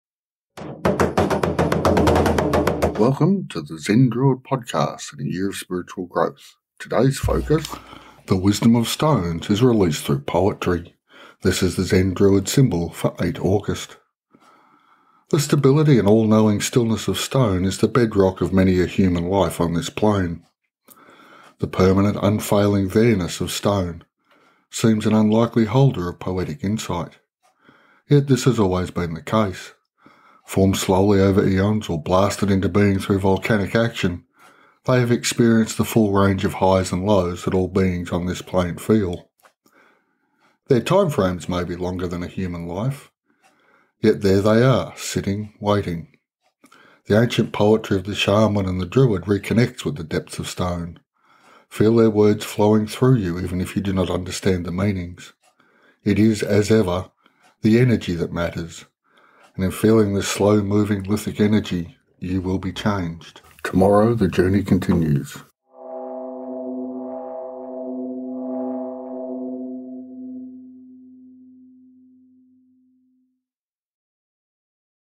The wisdom of stones is released through poetry
The solid dependable wonder of Nature's Way is released to you this day through poetry. Sit let the words come as you focus your mind upon standing stones. The wisdom is yours to share.